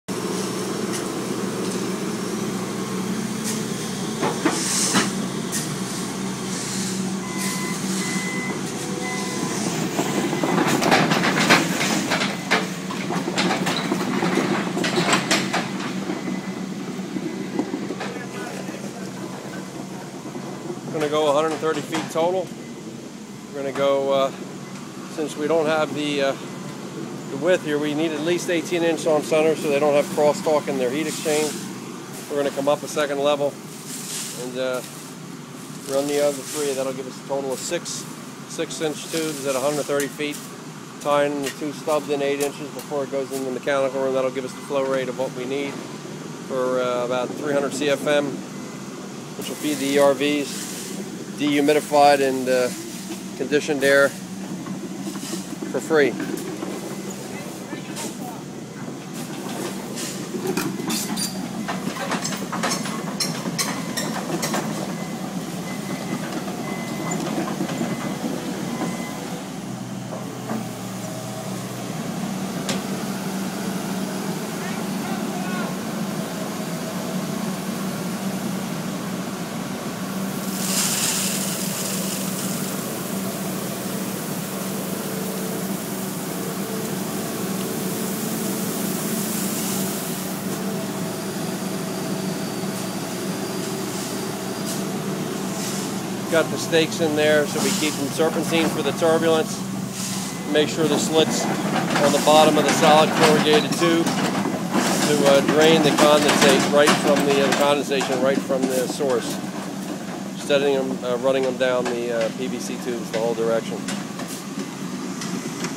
Earth Air Tube Installation Part I
Given the limiting constraints in our trench size, we opted to use a multi-level earth tube design that provided for immediate draining of moisture and condensation. This is a video of the lower level being installed.